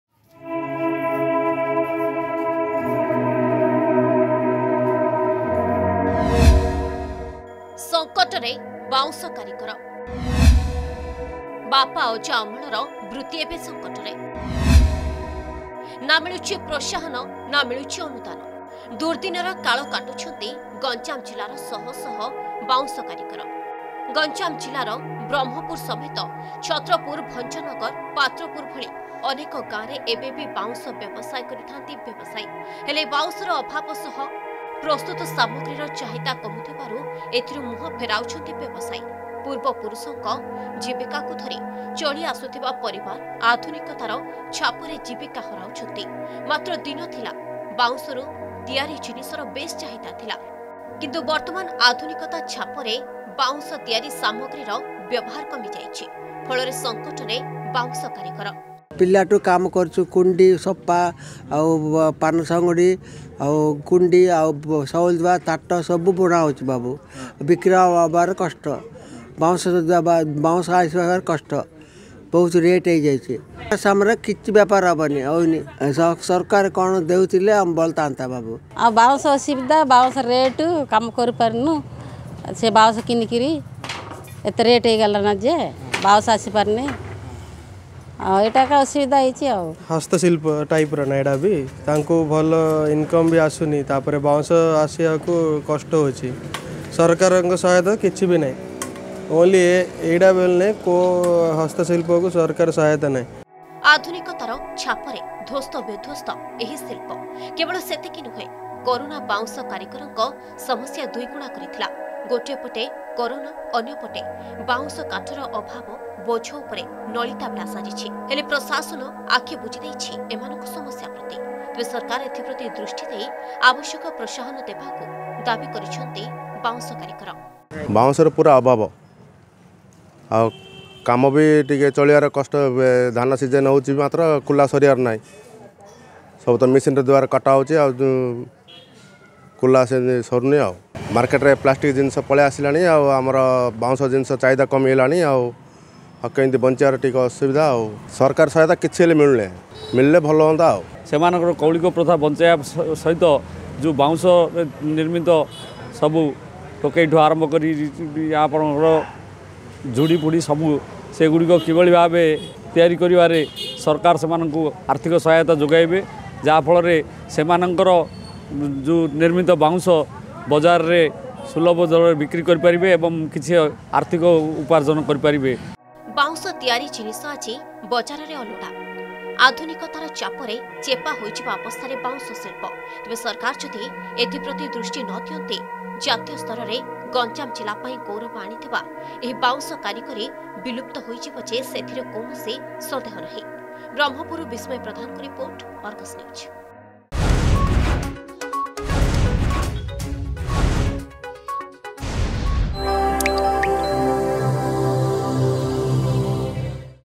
ସଙ୍କଟରେ ବାଉଁଶ କାରିଗର
ସଙ୍କଟରେ ବାଉଁଶ କାରିଗର । ବାପ ଅଜା ଅମଳର ବୃତ୍ତି ଏବେ ସଙ୍କଟରେ । ନା ମିଳୁଛି ପ୍ରୋତ୍ସାହନ ନା ମିଳୁଛି ଅନୁଦାନ । ଦୁର୍ଦ୍ଦିନରେ କାଳ କାଟୁଛନ୍ତି ଗଞ୍ଜାମ ଜିଲ୍ଲାର ଶହ ଶହ ବାଉଁଶ କାରିଗର ।